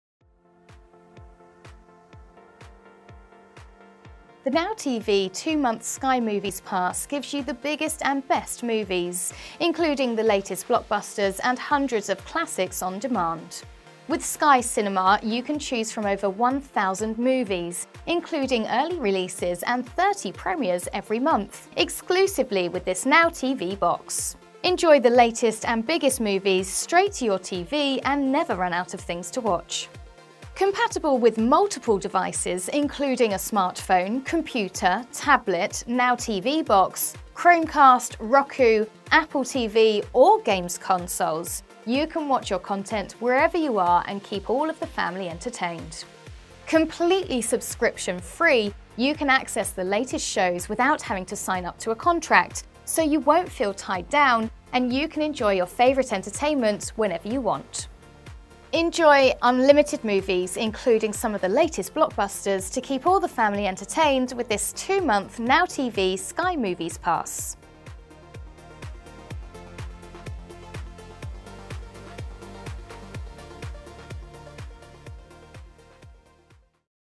NOW TV Sky Movies Pass - 2 Month | Product Overview | Currys PC World
Learn more about the NOW TV Sky Movies Pass.

The NOW TV two-month Sky movies pass gives you the biggest and best movies. Including the latest blockbusters and hundreds of classics on demand. With Sky cinema, you can choose from over one thousand movies, including early releases and 30 premieres every month, exclusively with this NOW TV box. Enjoy the latest and biggest movies straight to your TV and never run out of things to watch. Compatible with multiple devices, including a smartphone, computer, tablet, NOW TV box, Chromecast, Roku, Apple TV or games consoles, you can watch content wherever you are and keep all the family entertained. Completely subscription free, you can access the latest shows without having to sign up to a contract, so you won’t feel tied down and can enjoy your favourite entertainment whenever you want. Enjoy unlimited movies including some of the latest blockbusters to keep all the family entertained with this 2 month NOW TV Sky movies pass.